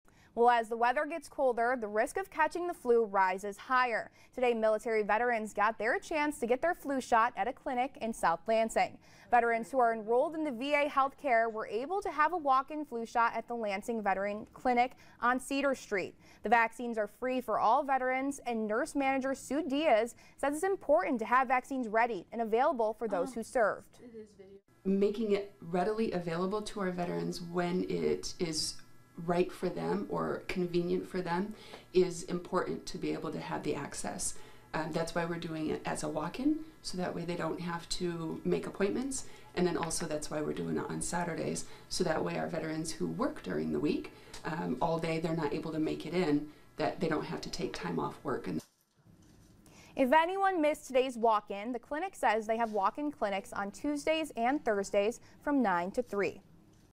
Lansing veterans receive free walk in flu shots from local clinic
If anyone missed Saturday’s walk-in, the clinic said they have walk-in clinics on Tuesdays and Thursdays